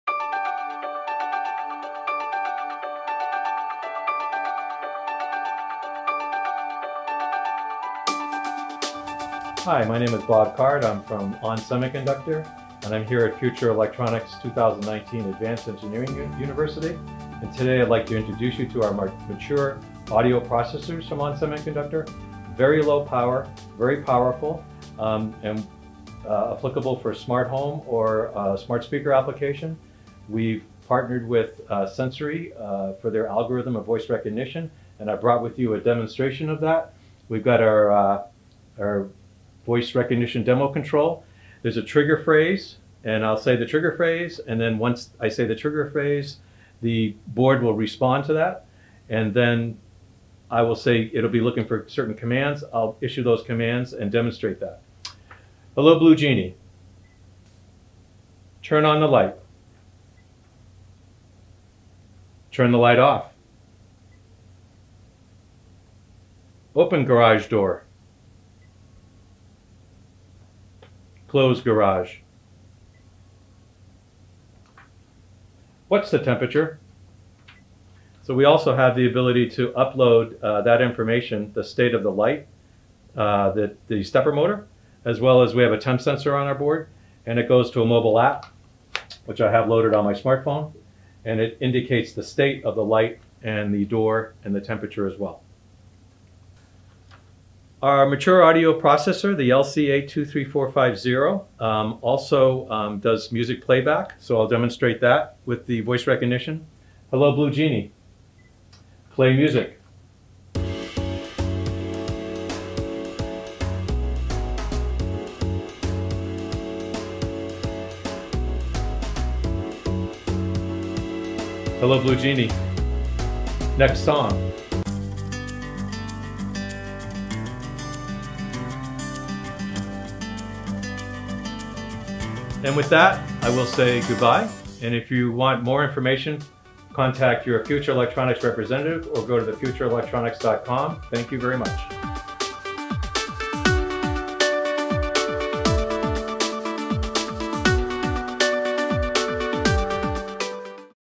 AEU2019: ON Semiconductor: Expanding Technology Horizons. Unravel the full board potential
Bob Card, Americas marketing manager ON Semiconductor, introduces us to the mature audio processor technology from On Semiconductor. During our recent Advanced Engineering University we caught up with Bob and took the opportunity to see a quick demonstration of how voice recognition technology can work in a smart home or smart speaker applications.

ON Semiconductor has partnered with Sensory for their algorithm of voice recognition for this demo. Learn more about this by contacting your local Future Electronics sales representative or by clicking the link below to learn more about the evaluation board available.